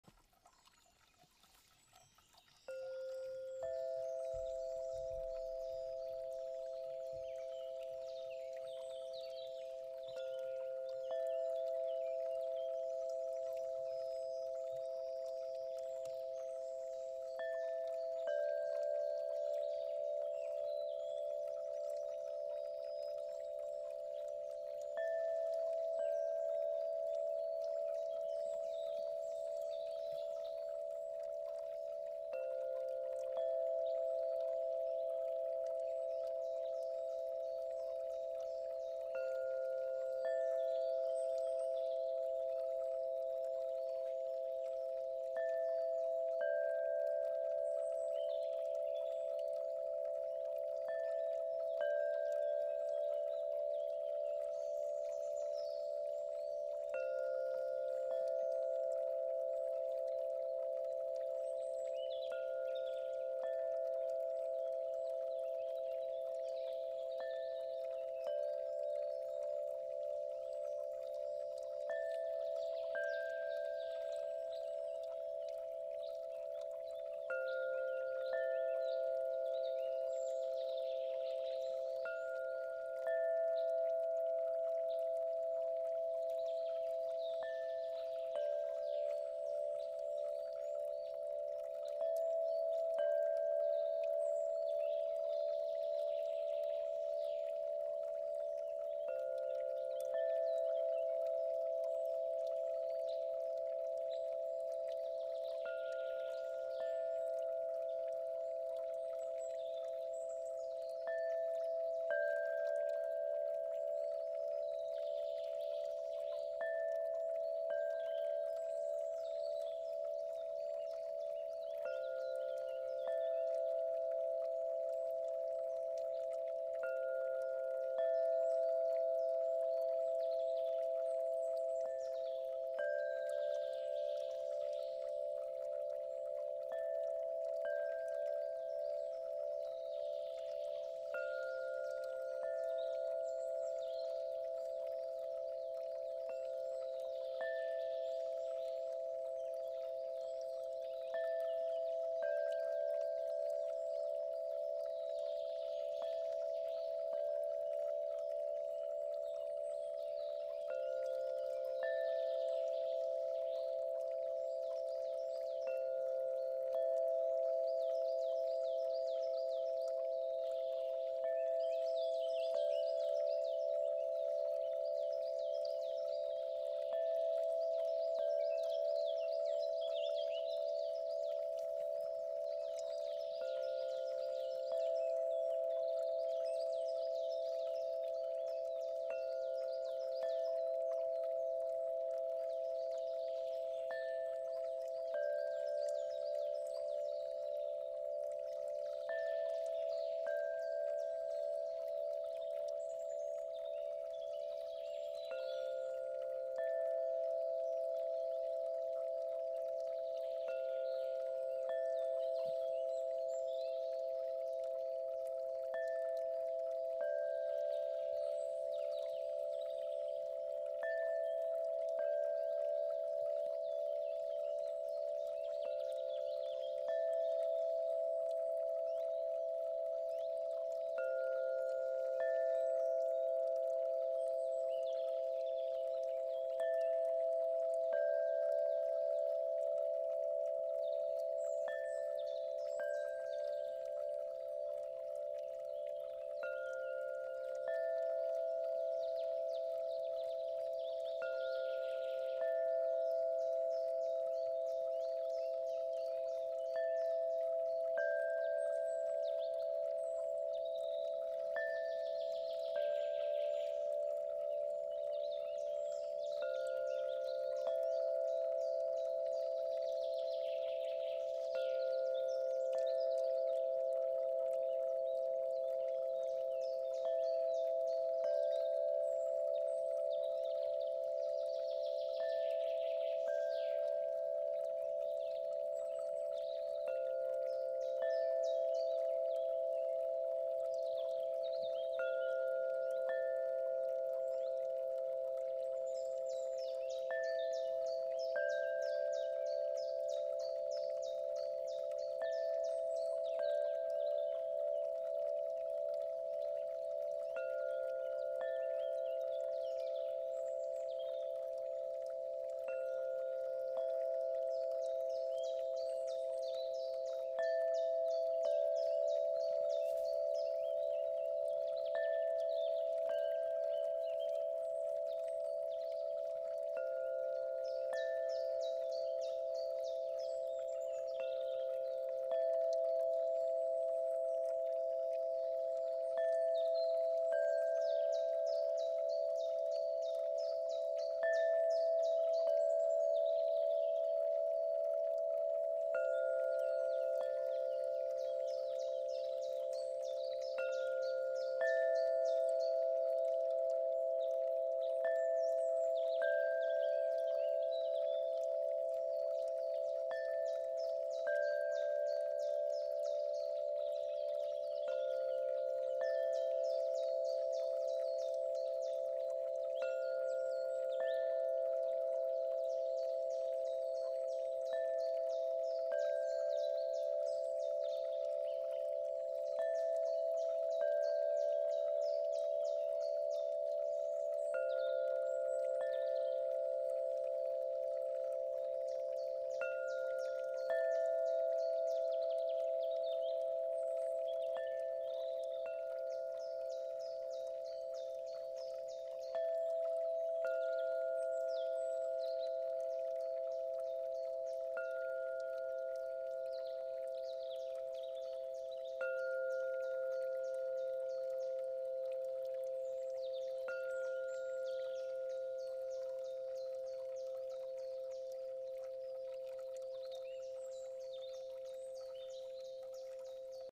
Kristall Harfen Meditation/ Crystal harp meditation 01
Zu dem plätschern von der Quelle klarem Wassers, gesellt sich die Kristallharfe mit ihren reinen Klängen, harmonisch in die Landschaft ein.    Danke!